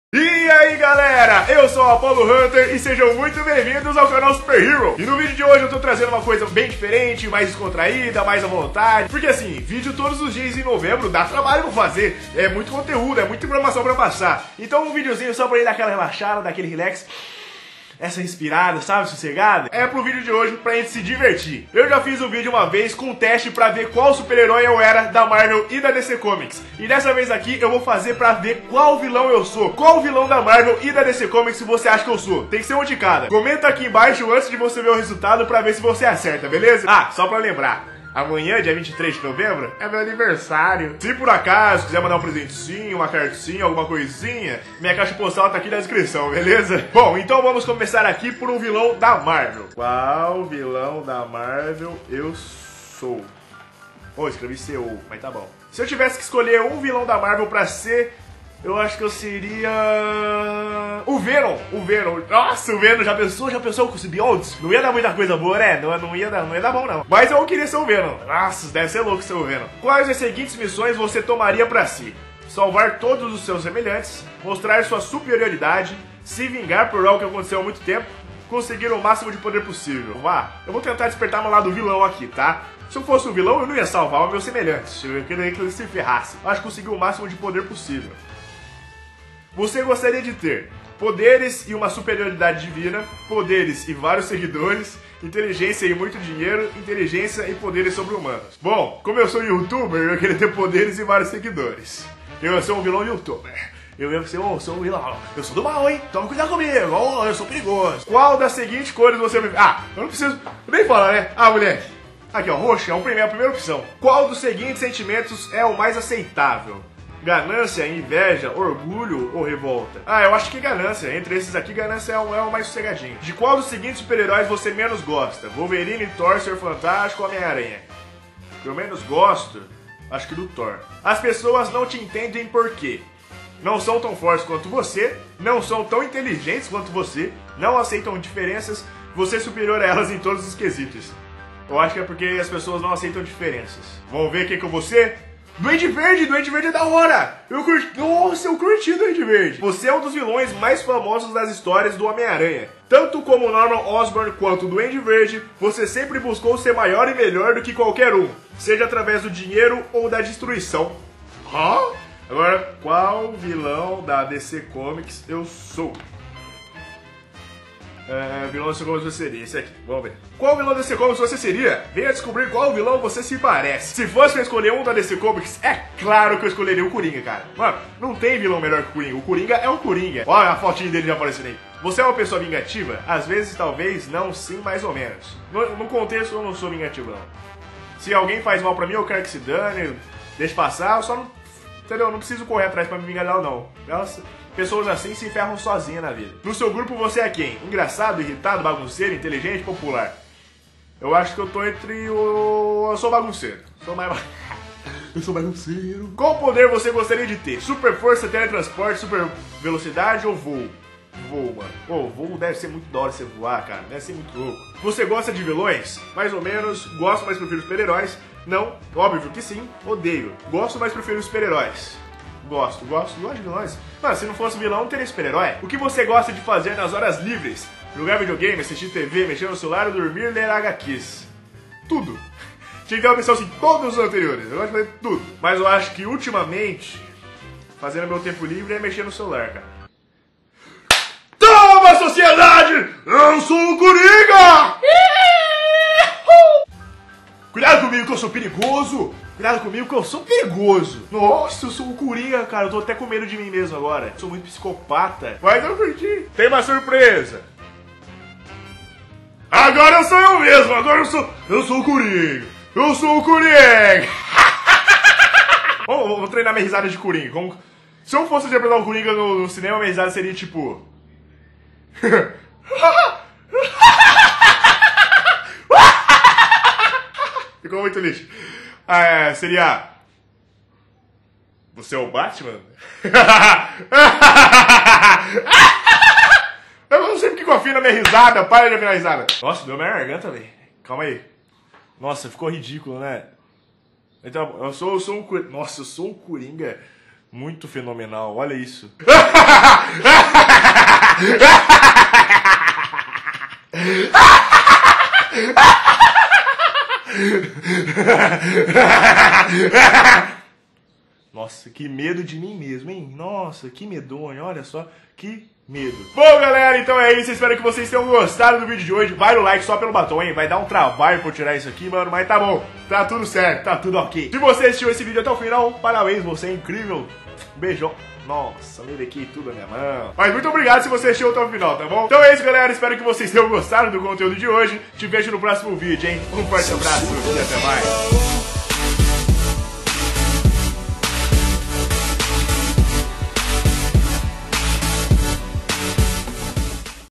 QUAL VILÃO? 😈😱
Vídeo diferente pra vocês!

Um quiz para sabermos qual super vilão da Marvel e da DC Comics eu sou!

ME MANDEM COISAS NA CAIXA POSTAL ❤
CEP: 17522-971
CX: 921

VENHA CONVERSAR COMIGO!